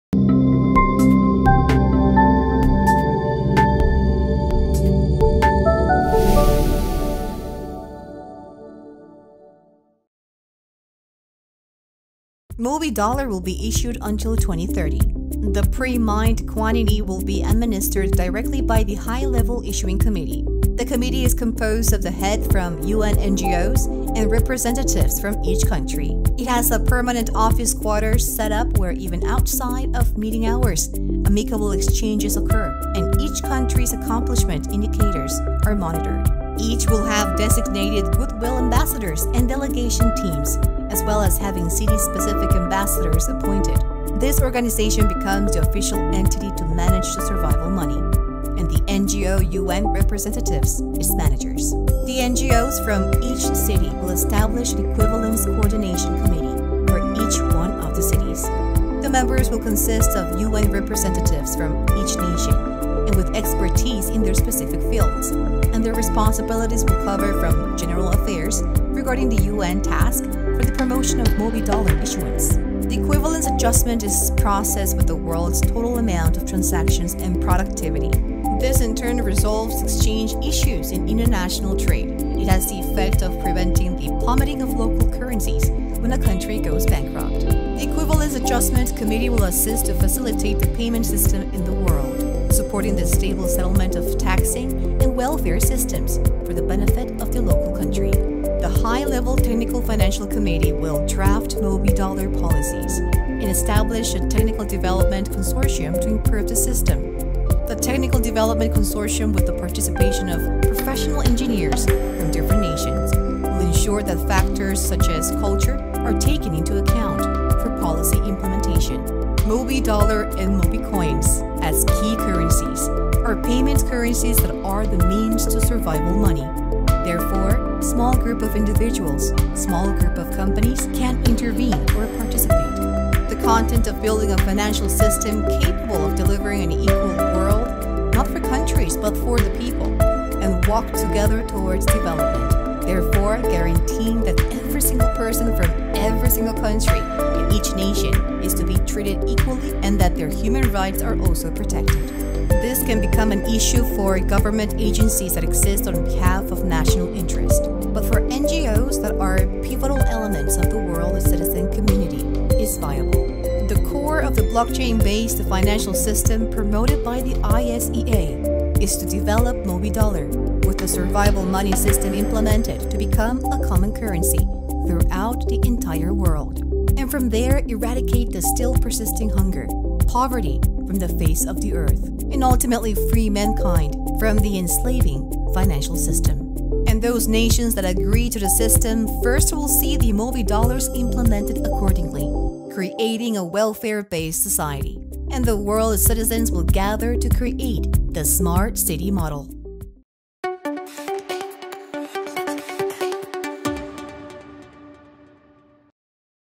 Blockchain Finance : Mobidollar - Part 6) モビドルシステムのロードマップ
モビドルシステムのロードマップ
モビドル（MOBI Dollar）は2030まで全量放出されます。Pre-mining数量は高いレベルの放出調整委員会で管理します。
放出調整委員会はモビドルを支持する各国の国連NGOの代表たちと各国の代表団として構成されます。この機関は、生存マネーを担当する実際の運営組織となり、NGO国連の代表が生存マネーを運営する事になります。
各都市のNGOは都市別に等価調整委員会を構成する。
高いレベルの技術・金融委員会はモビドルの政策を準備し、システムの技術的な補完のために技術開発コンソーシアムを傘下に置く。